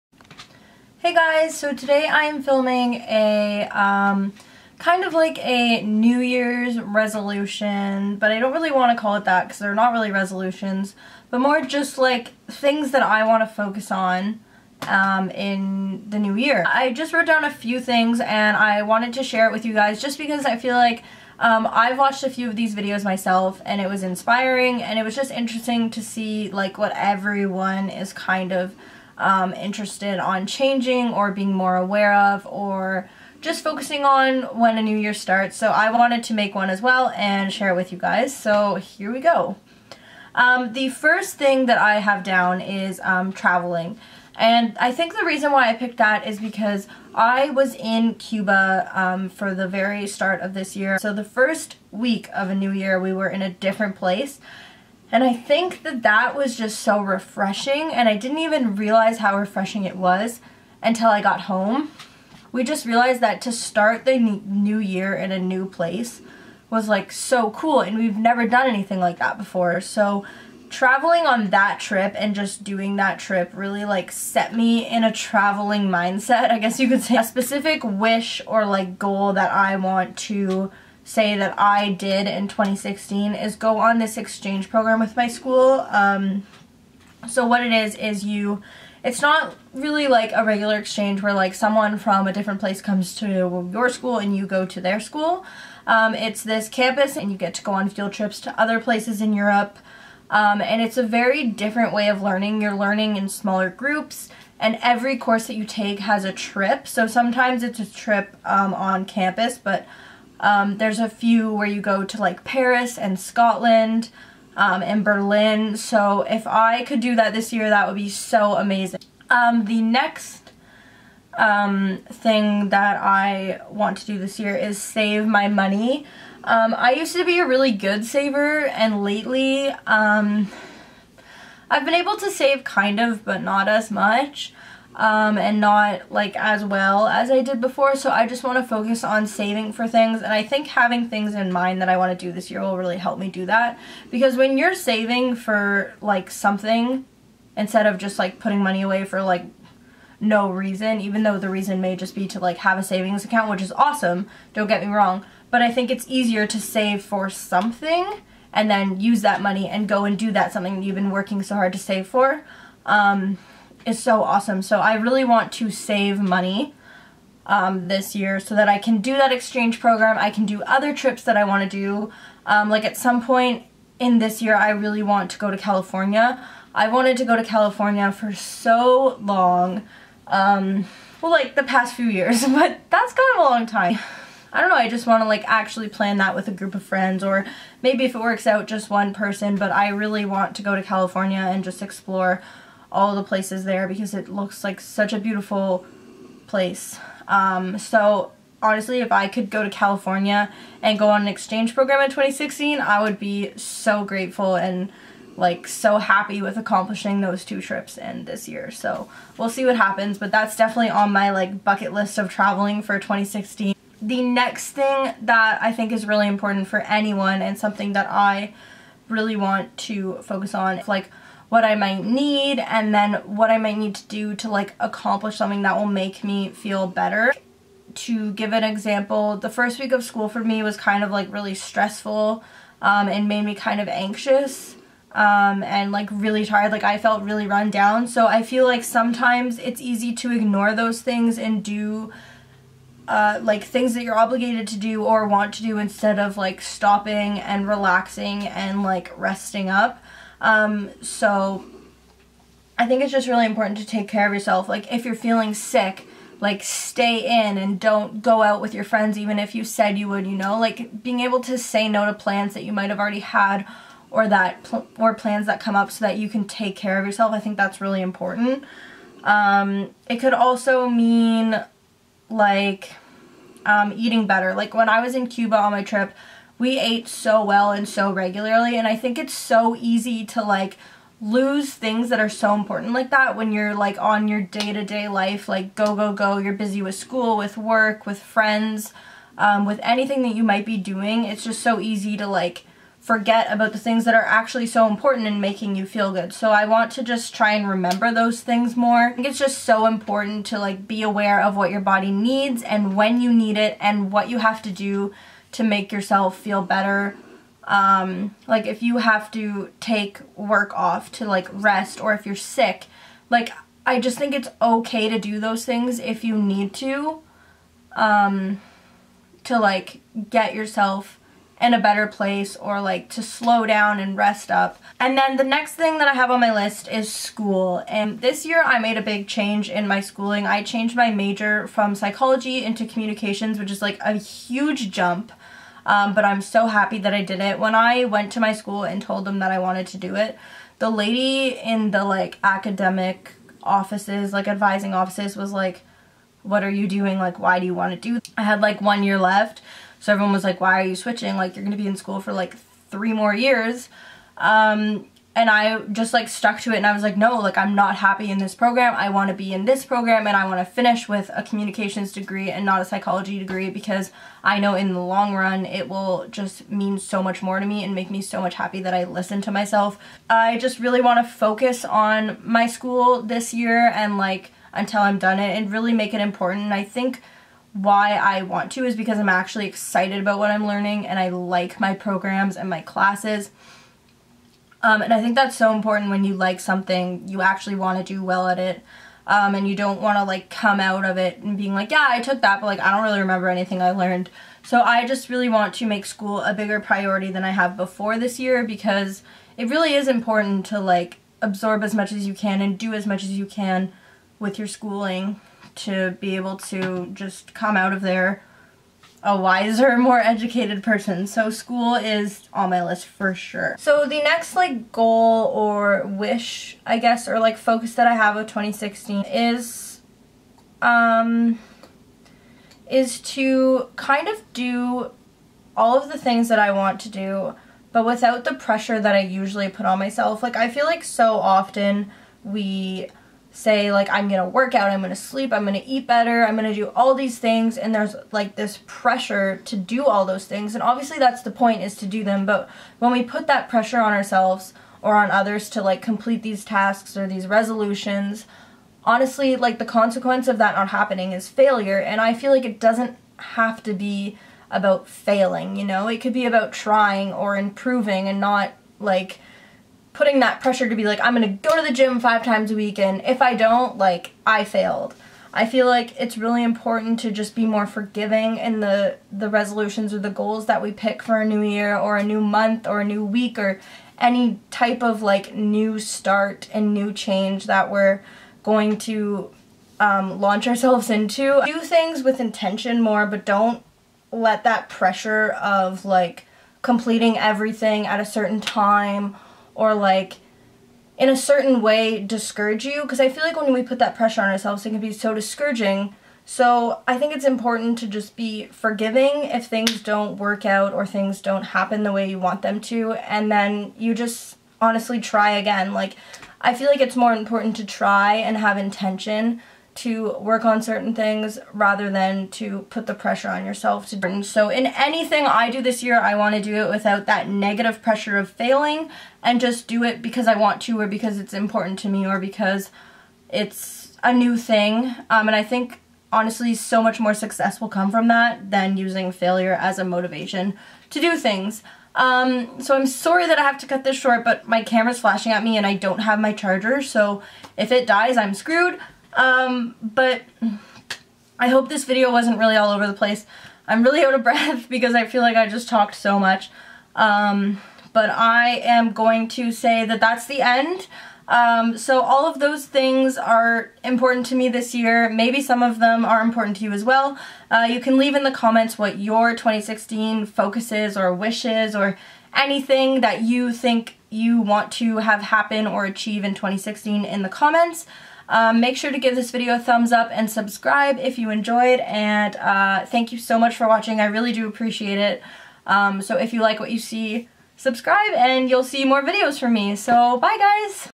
My Intentions | 2016
Hey guys! Happy New Year!!

I know this may be considered late for a news years video, but really it can apply at any point! In this video Im sharing my goals and wishes for the new year! What are some of yours?